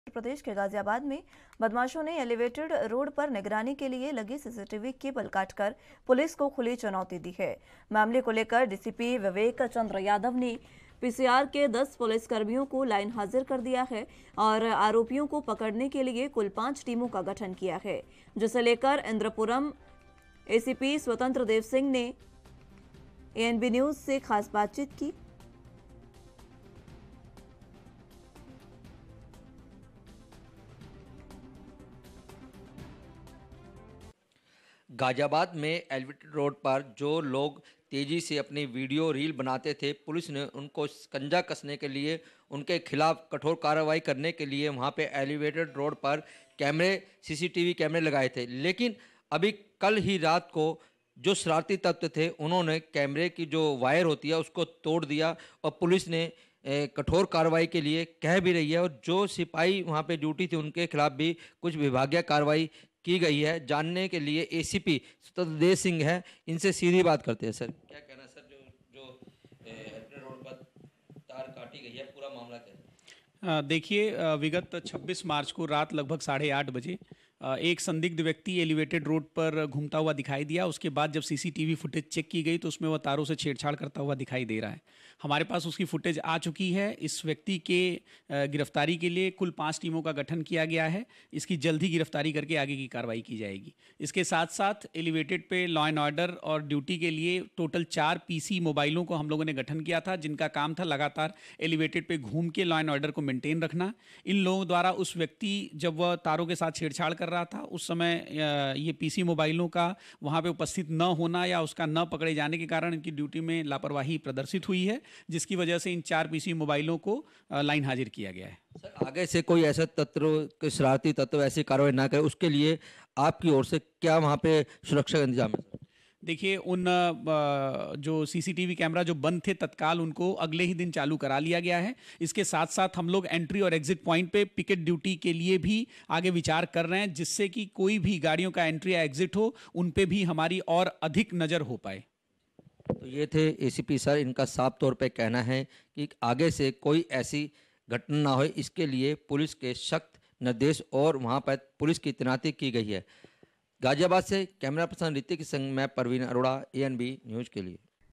ACP स्वतंत्र देव सिंह से ANB न्यूज़ की खास बातचीत | ANB NEWS
बदमाशों की पुलिस को खुली चुनौती
निगरानी के लिए लगे CCTV का काटा वायर
आरपियों को पकड़ने के लिए कुल 5 टीमों का गठन
ACP स्वतंत्र देव सिंह से ANB न्यूज़ की खास बातचीत